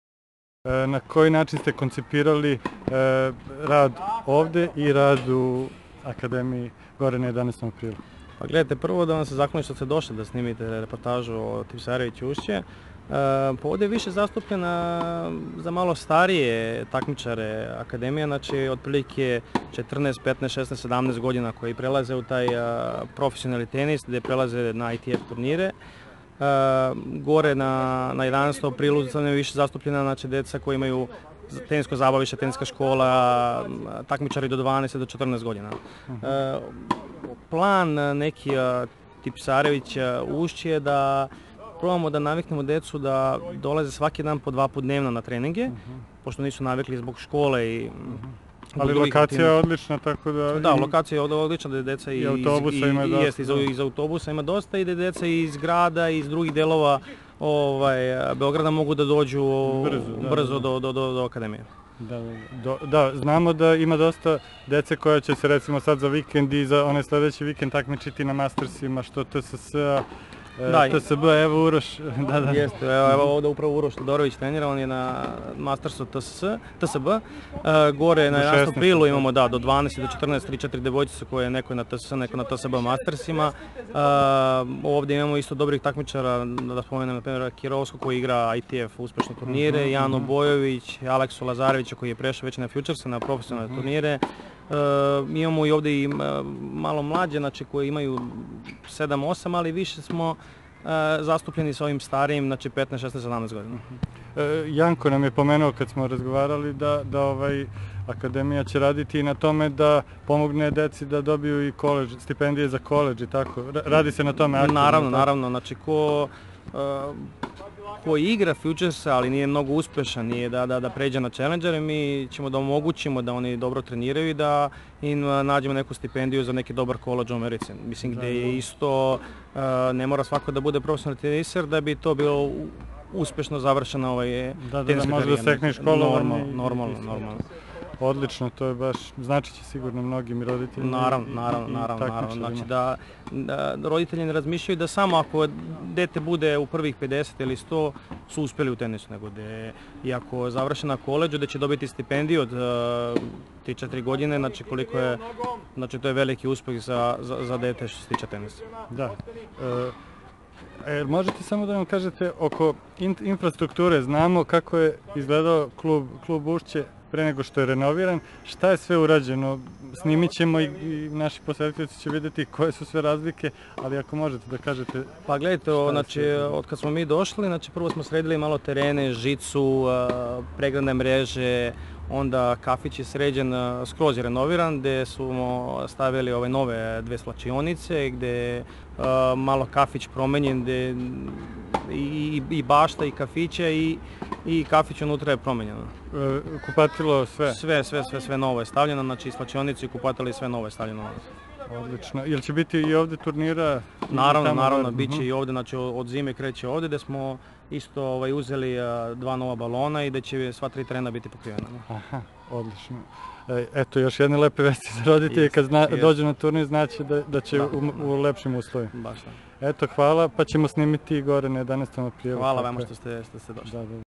Nemanja Plavšić o aktivnostima u TA Tipsarević Ušće
Менаџер клуба Немања Плавшић објаснио нам је какав је концепт рада у академији и како је конципиран рад код „11. априла“ а како код ТЦ Ушће. Попричали смо и о помоћи академије својим полазницима да добију стипендије на америчким колеџима, о чему нам је недавно причао и оснивач академије Јанко Типсаревић